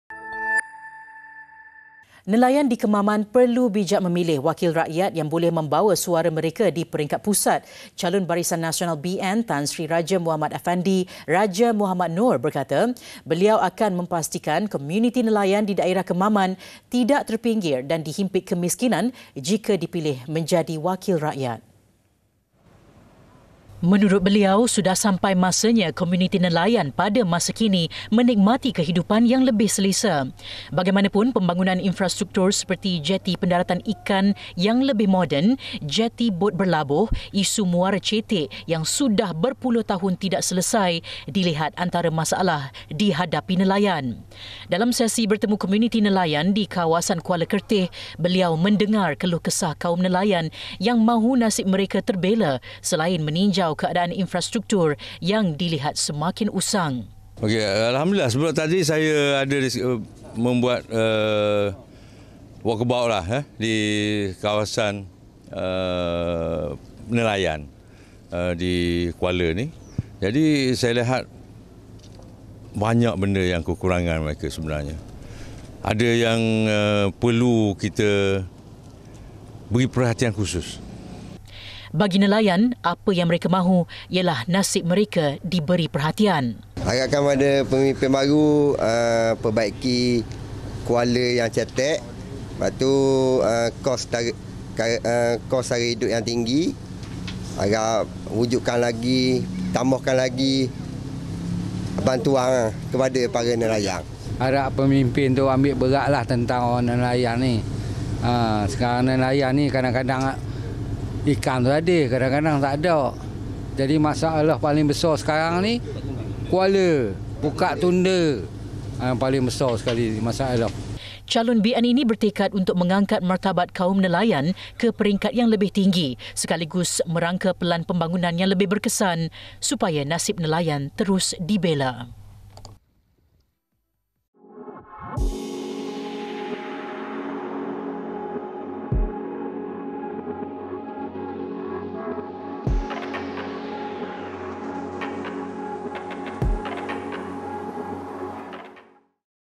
PRK KEMAMAN | Komuniti Nelayan Di Kemaman Perlu Bijak Memilih Wakil Rakyat
Nelayan di Kemaman perlu bijak memilih wakil rakyat yang boleh membawa suara mereka di peringkat pusat.